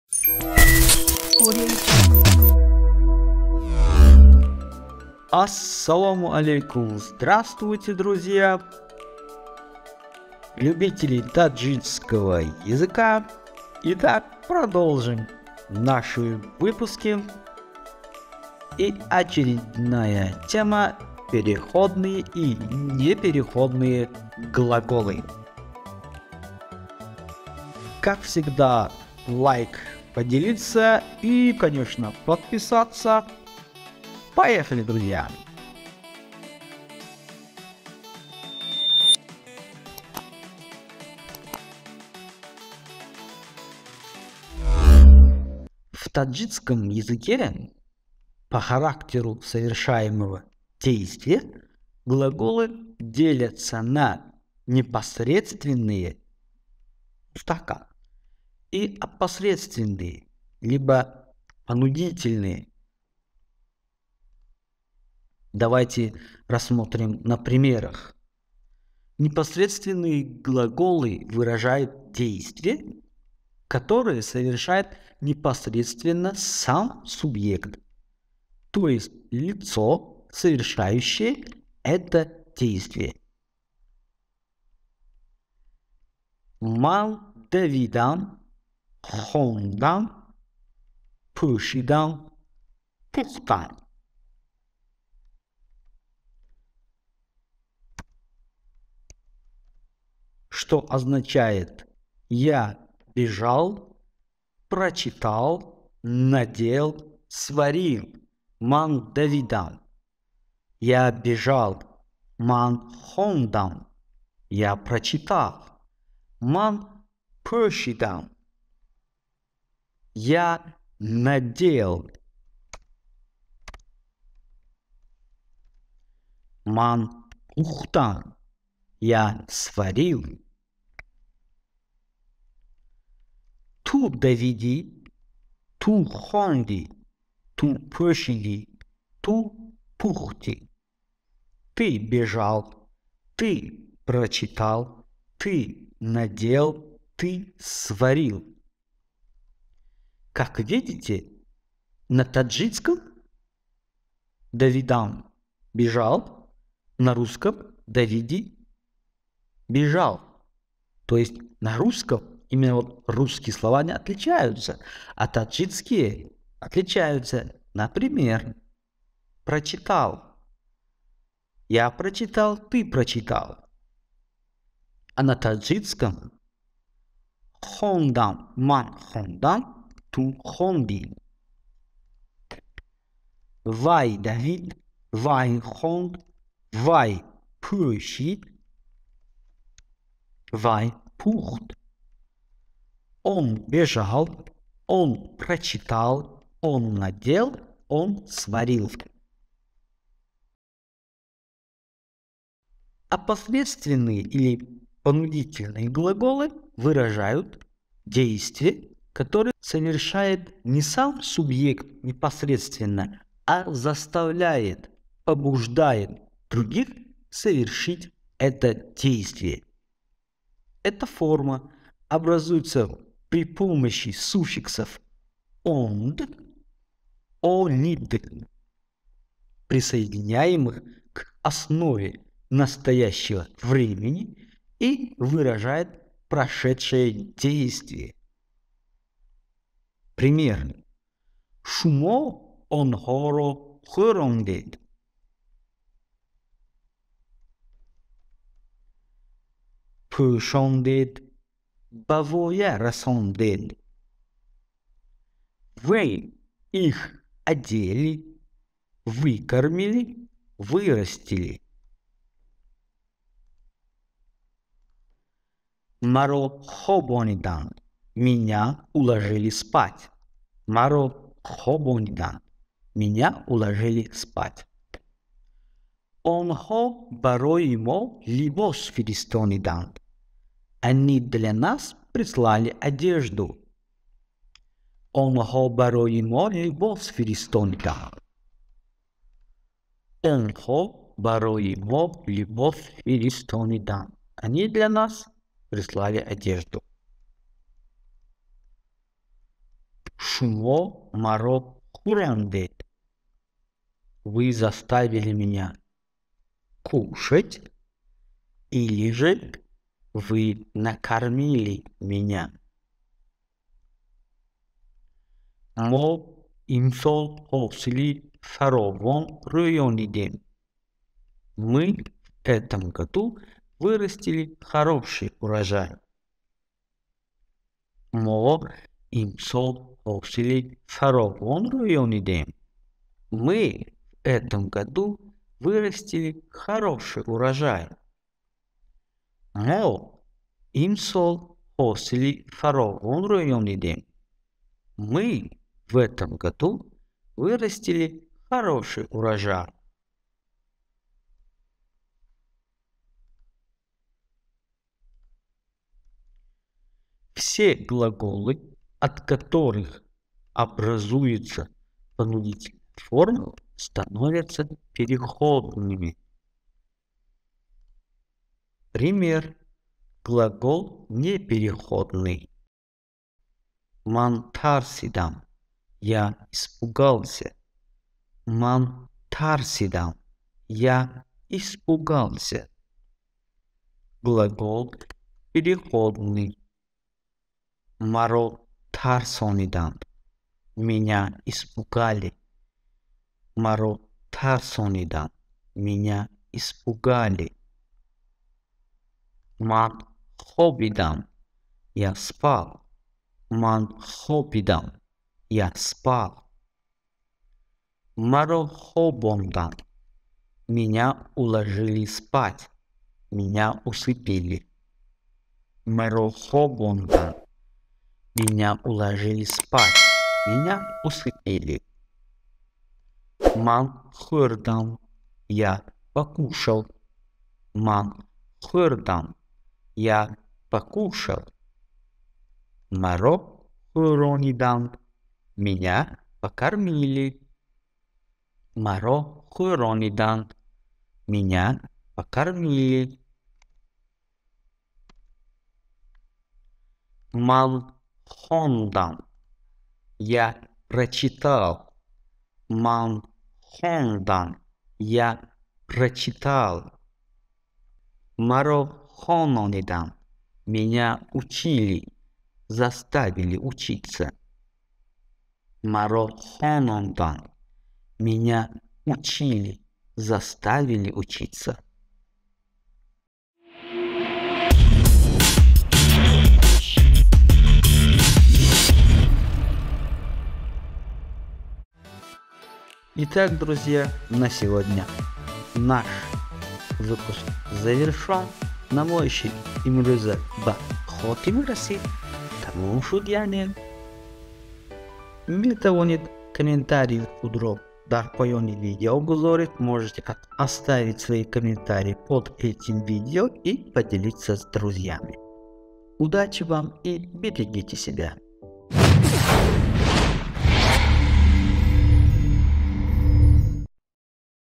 Изучение таджикского языка - Переходные и неперехоные глаголы - продолжение
Надеюсь, что видео вам понравилось, и если вам есть, что сказать, то оставляйте комментарии внизу! Умедворам, ки видео ба шумо маъқул шуд ва агар гуфтание шумо дошта бошед шарҳҳои худро гузоред!

В таджикском языке по характеру совершаемо¬го действия глаголы делятся на непосредственные (мустахкам) и опосредственные (понудительные).

Непосредственные глаголы выражают дейст¬вие, которое совершает непосредственно сам субъ¬ект, т. е. лицо, совершающее это действие. На¬пример: Ман Давидам, хондам, пушидам, пухтам.

— Я бежал, прочитал, надел, сварил. Ту давидй, хондй, пушидй, пухтй.— Ты бежал, прочитал, надел, сварил. Вай давид, хонд, пушид, пухт.— Он бежал, прочитал, надел, сварил.